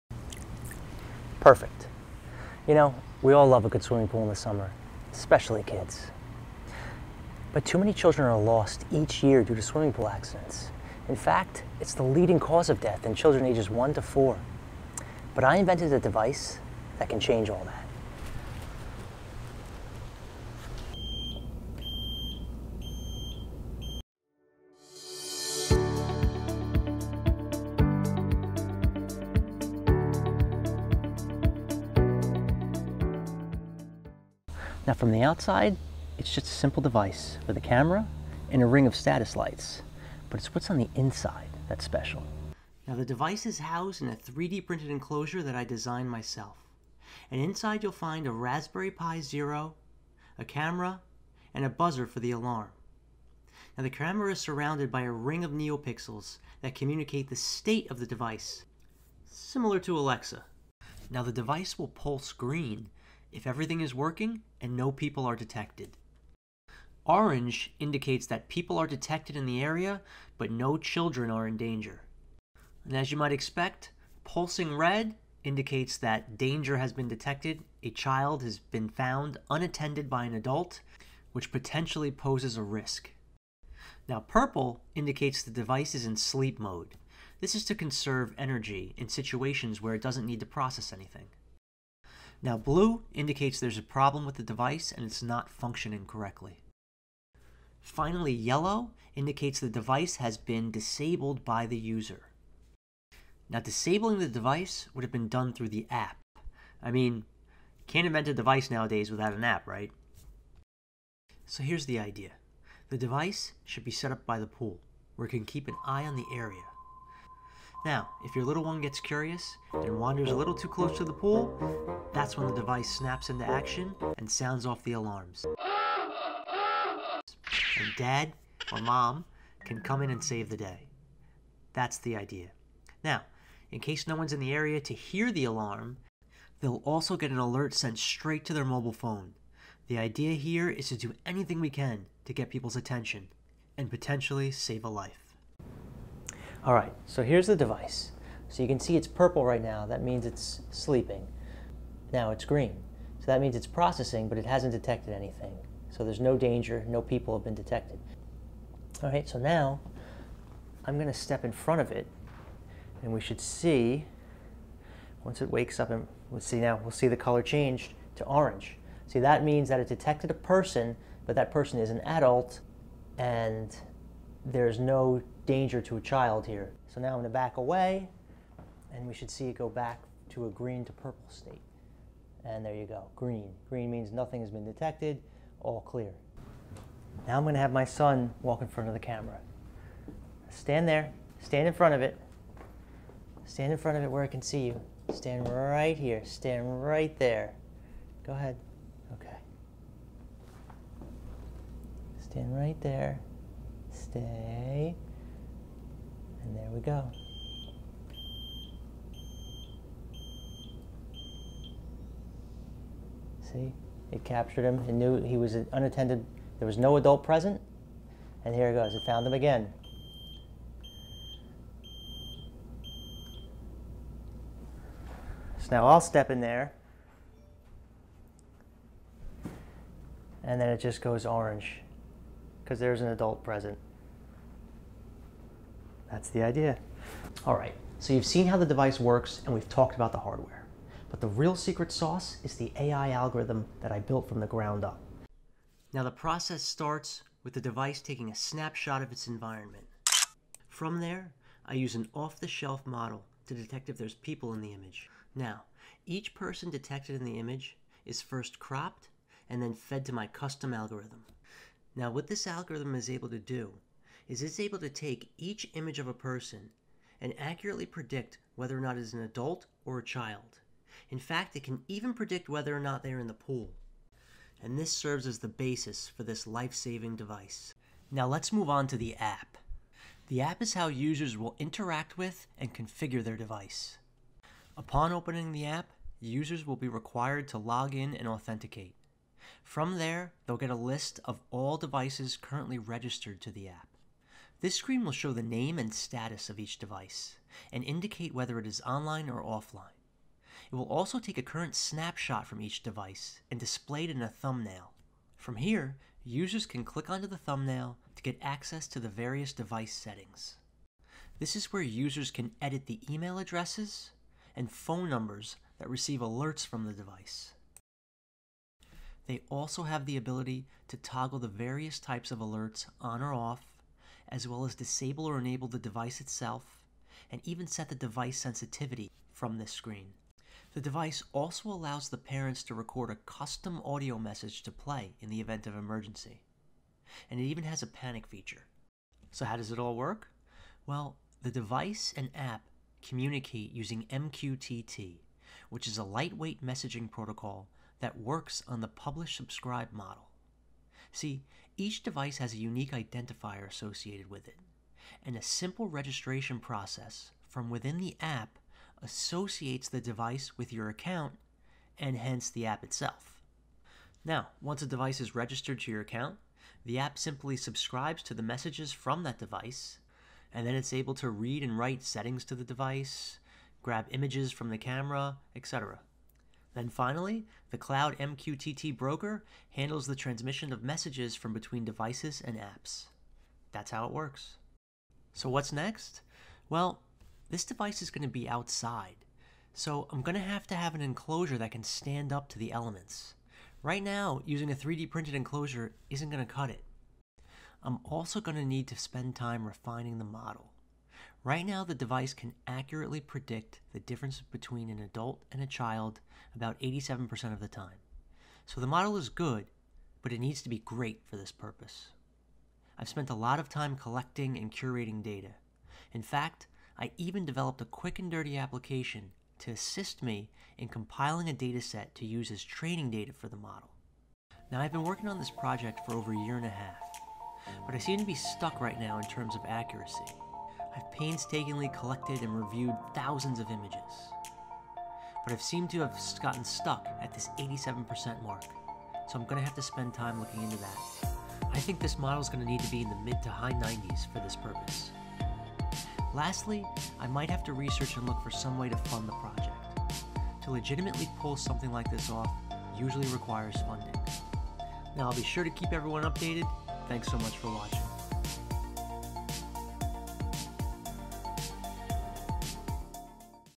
My AI powered life saving invention
An in-depth look at a device I invented using artificial intelligence that can potentially save children from downing in swimming pools.